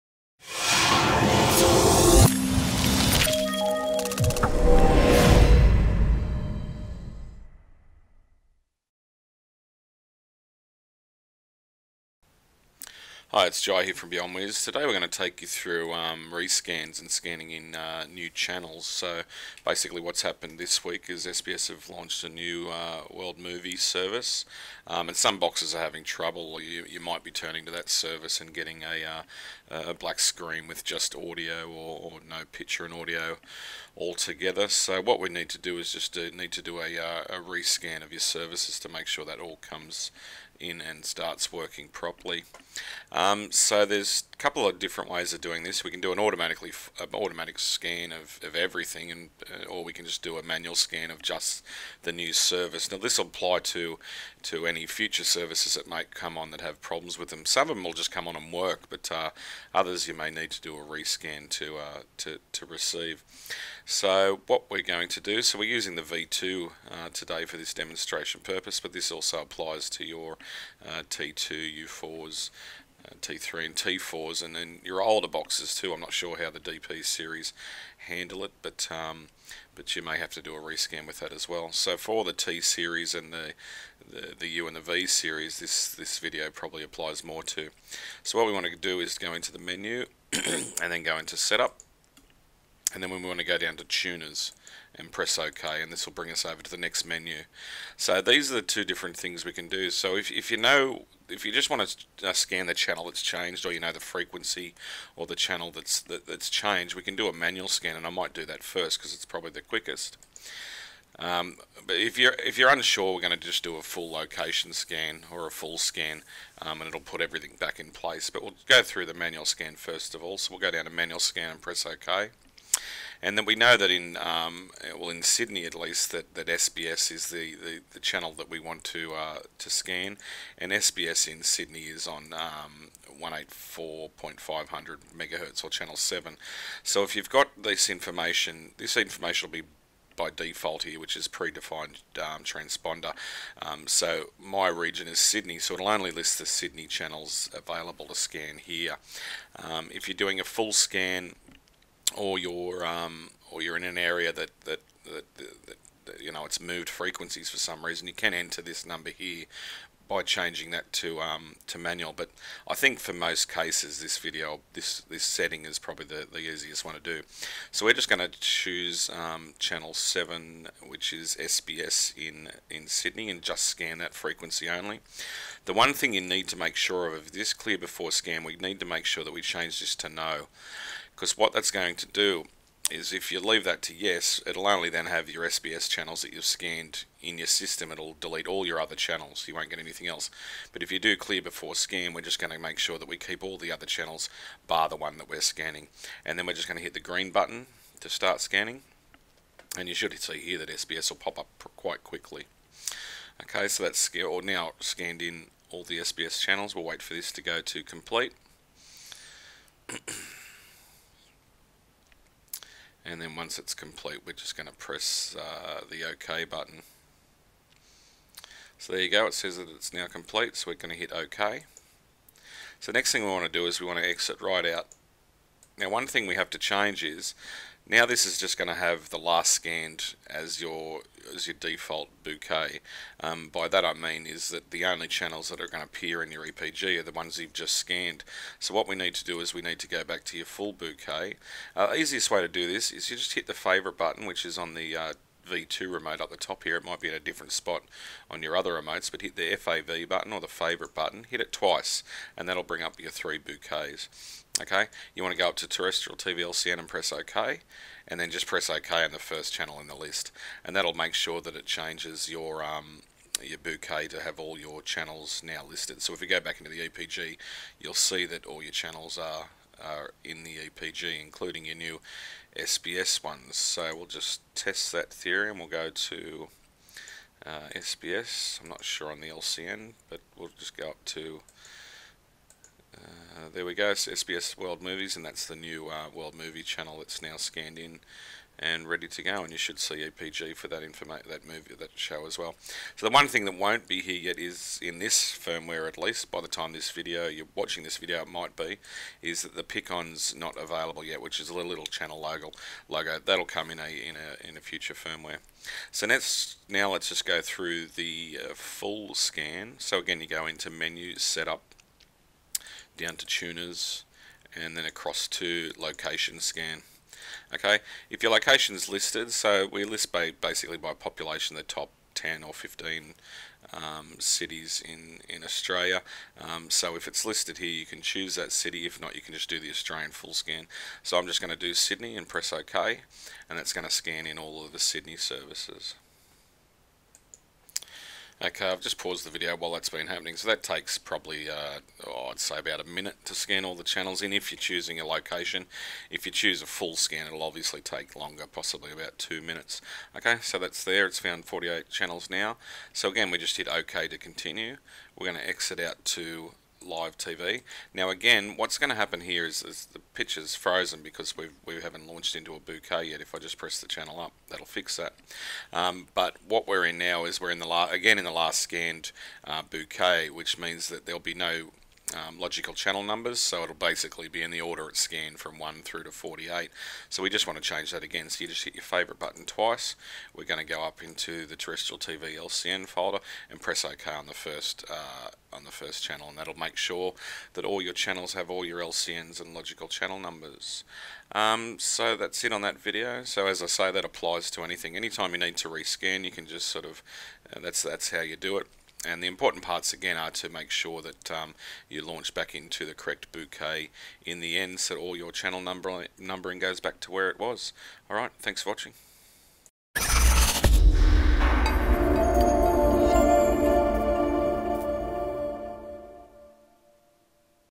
Re-scanning the Channels on the Beyonwiz V2, U4, T2, T3, T4
This video takes you through doing a fresh channel scan without the need for a factory reset. Recently SBS World Movies started broadcasting on free to air in Australia, this video will show you how to scan in any new channels.